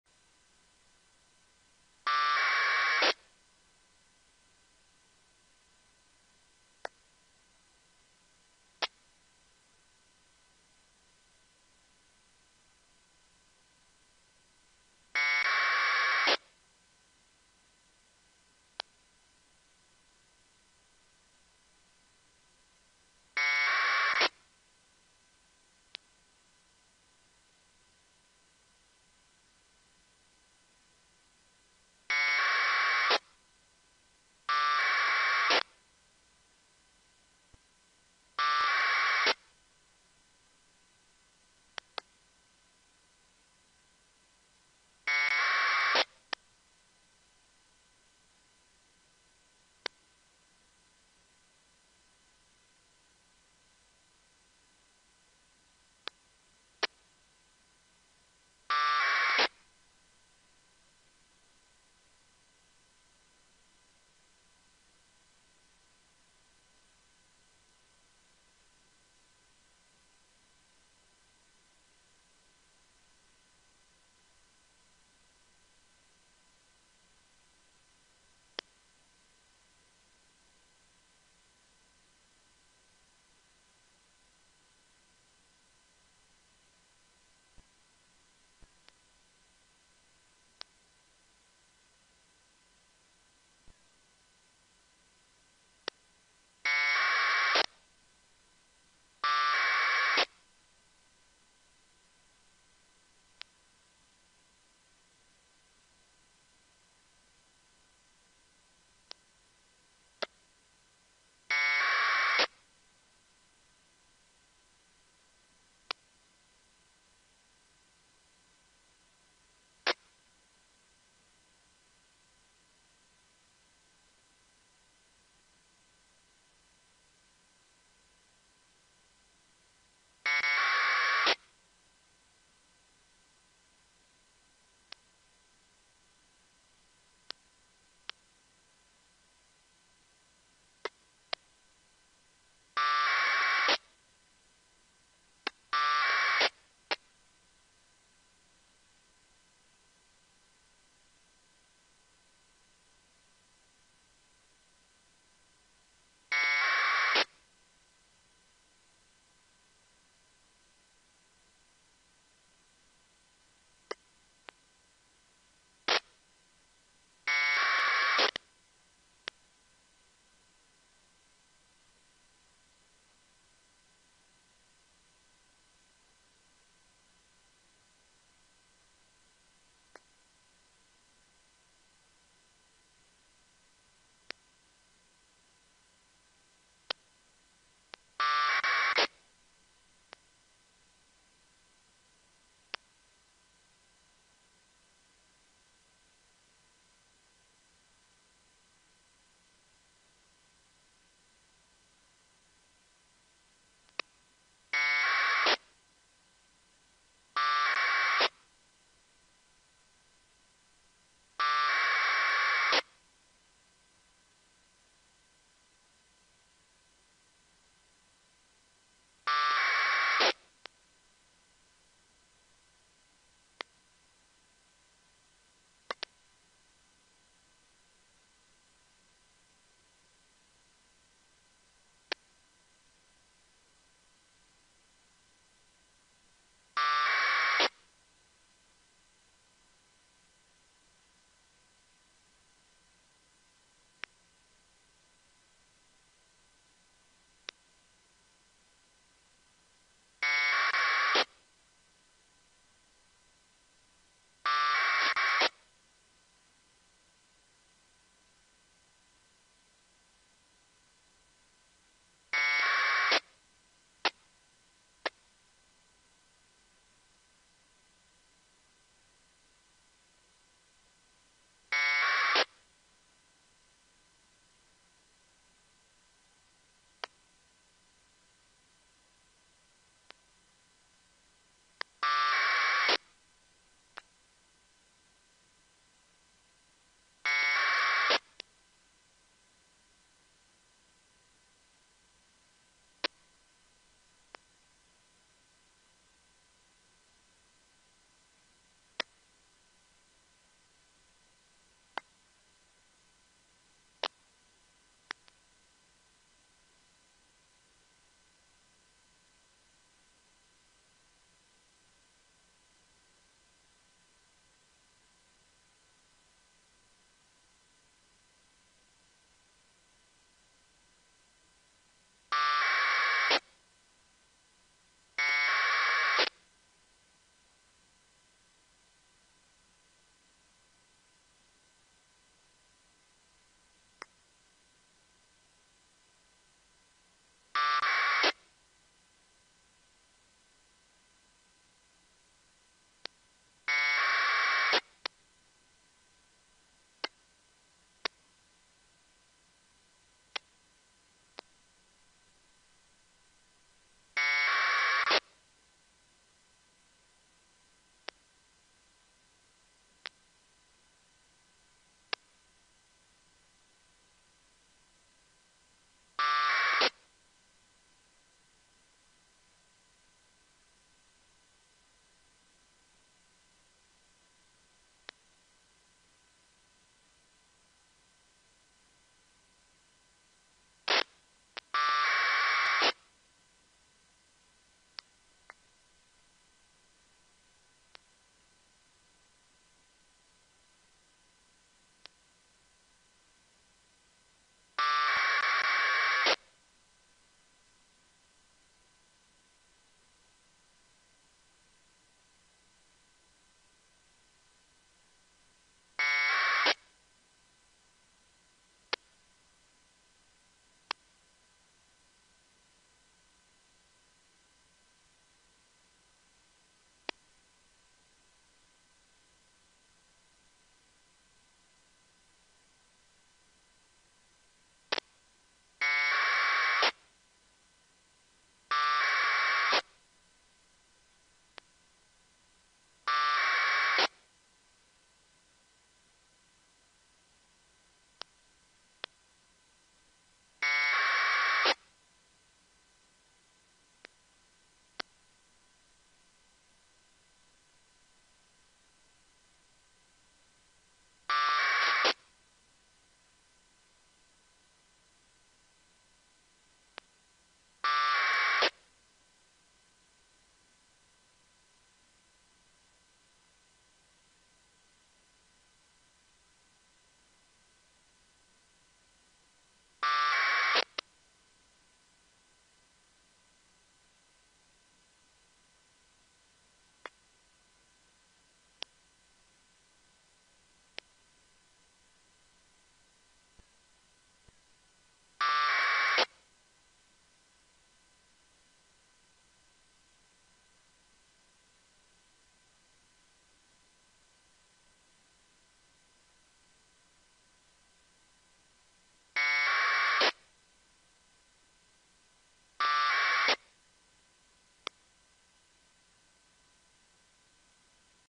Using AFSK1200 Decoder for Radio Propagation
Using my RTL-SDR Card running SDR# tuned to the APRS frequency 144.390 MHZ NFM, Filter bandwidth set at 11400, squelch set at 50 and running AFSK1200 Decoder, I have built a system for decoding local radio propagation.